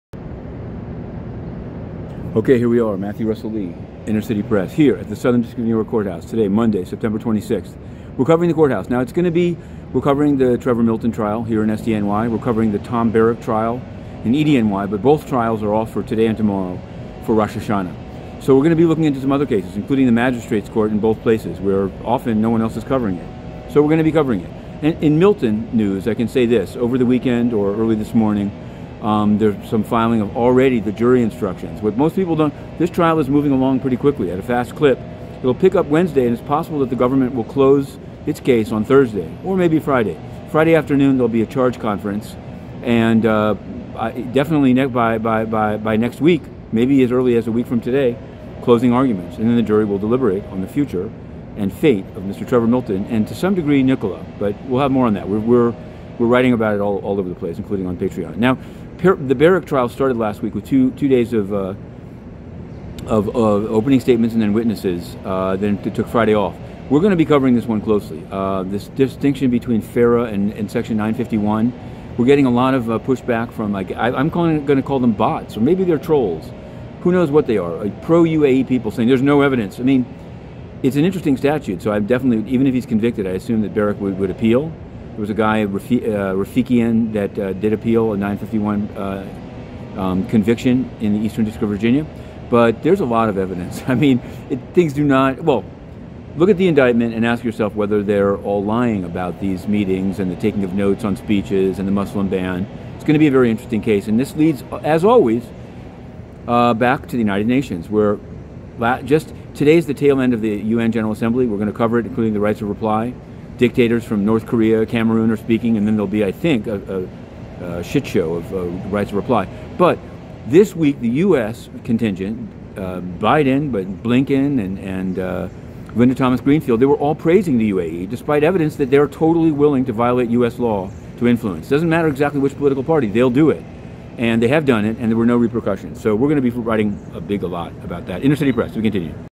VLOG: US v Milton pause; US v Barrack trolls EDNY, @USUN pro UAE silent on UN censors, UNGA end
VLOG: US v. Milton jury instructions; US v. Barrack trolls EDNY, briber UAE celebrated by
silent on UN censorship, end of UNGA replies CMR NK